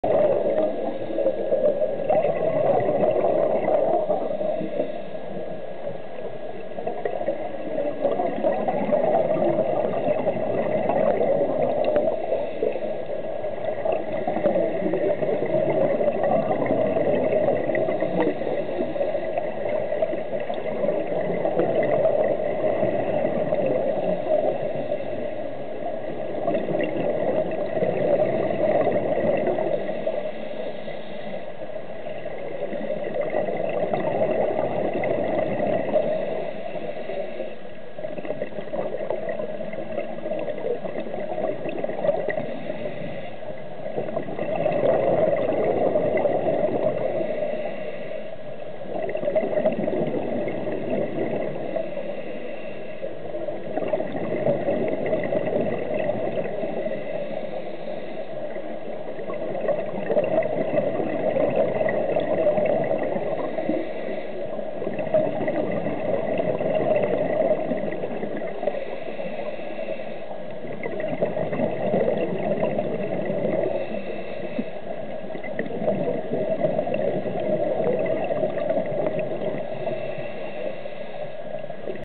Diving on the aircraft in Curacao 1
My son and daughter diving along an aircraft